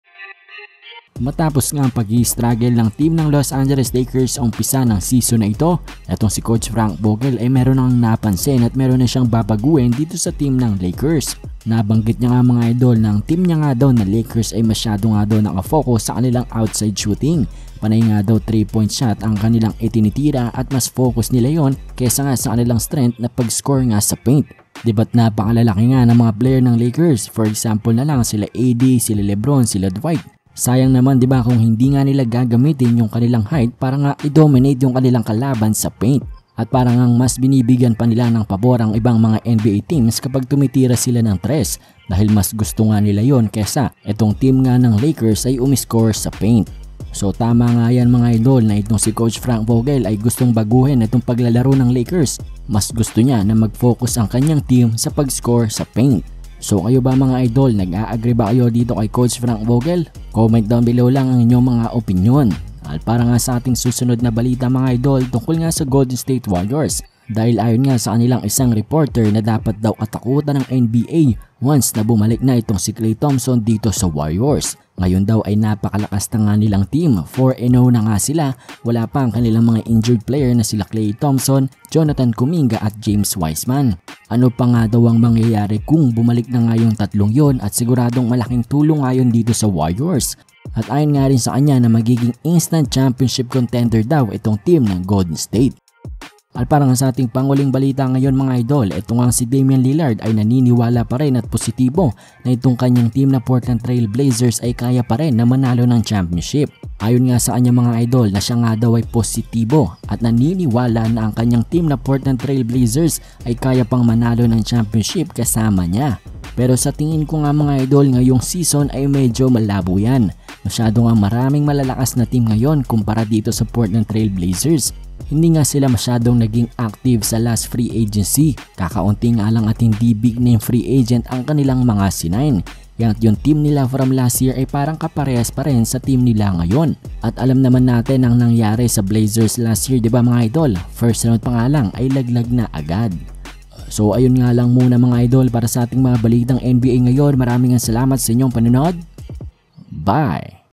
Frank Vogel May Babaguhin Sa Lakers! Warriors Kaka-Takutan Ng NBA! Lillard Hindi Aalis Sa Blazers!
Lakers May Babaguhin Na Sa Team Nila!  Warriors Kaka-Takutan Ng NBA! Lillard Hindi Aalis Sa Blazers!